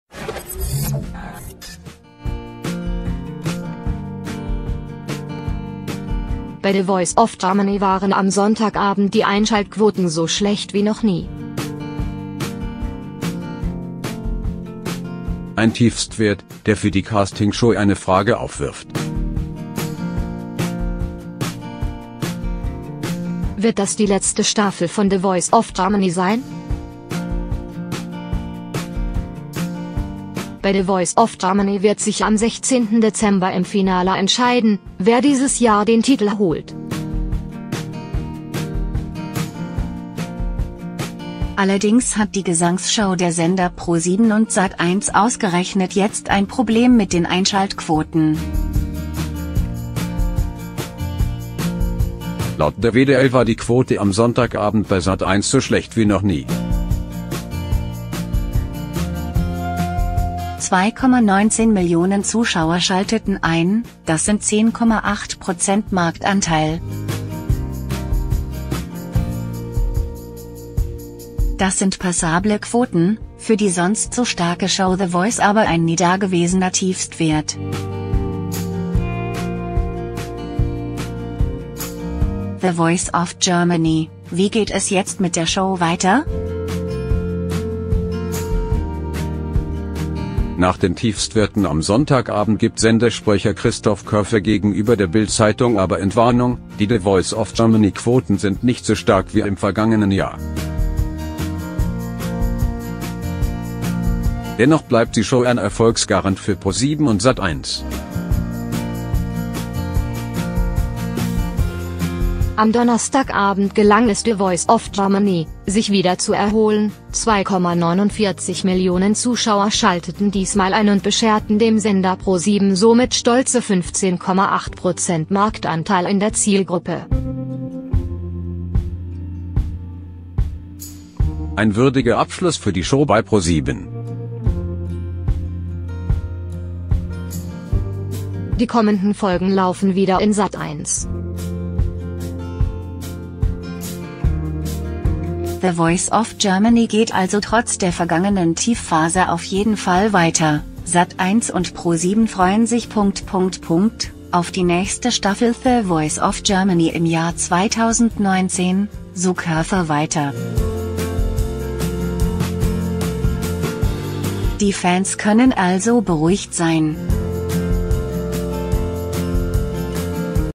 Nach Quotentief: Wie geht es mit „The Voice of Germany“ weiter? - FPM
Immagine della licenza CC:
Https: //Plaughter.com